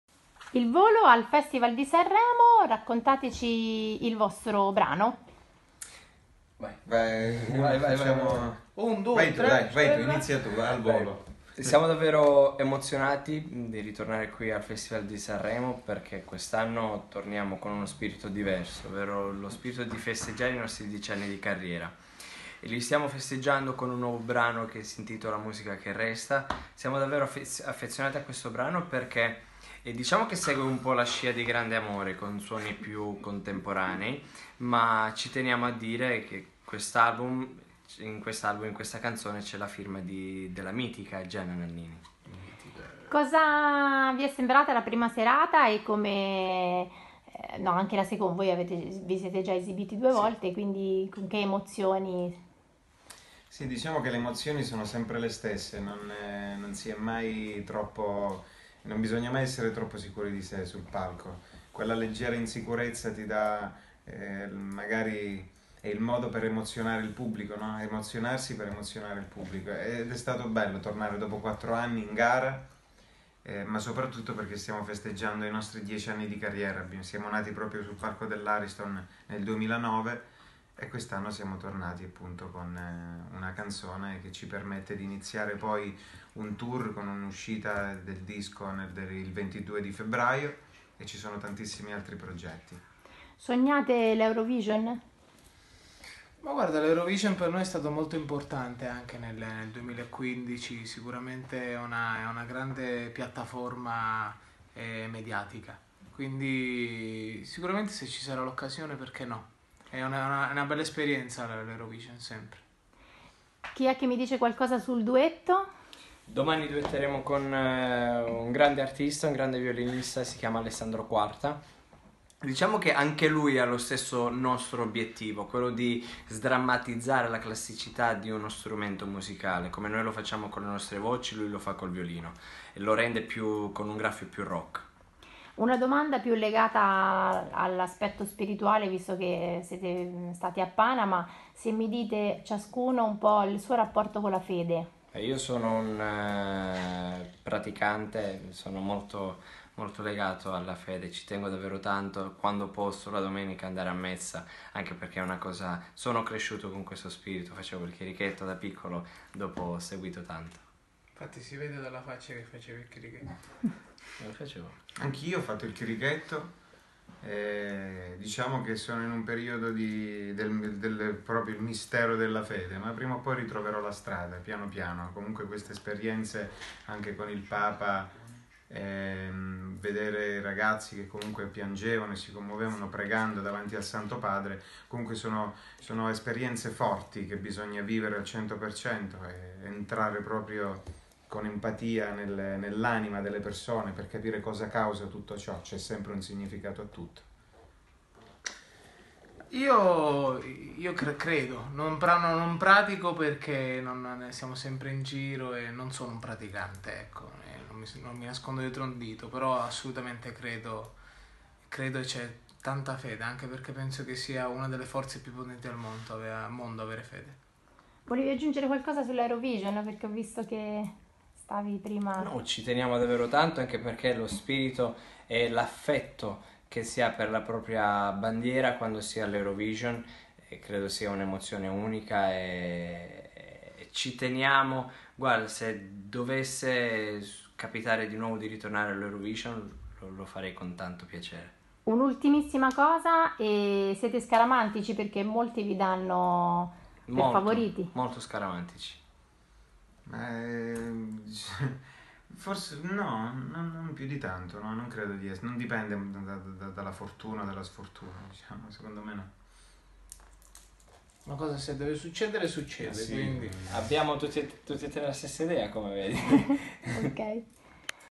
Sanremo 2019. Il Volo e quel rapporto con la fede che non ti aspetti
Ai microfoni di Serena Il Volo che parlano a cuore aperto dell'esperienza al Festival e del loro rapporto con la fede. Parole molto profonde che dovrebbero far pensare i giovani, oggi così lontani.

Puoi seguire lo Speciale in tempo reale fino alla fine del Festival di Sanremo 2019 a questo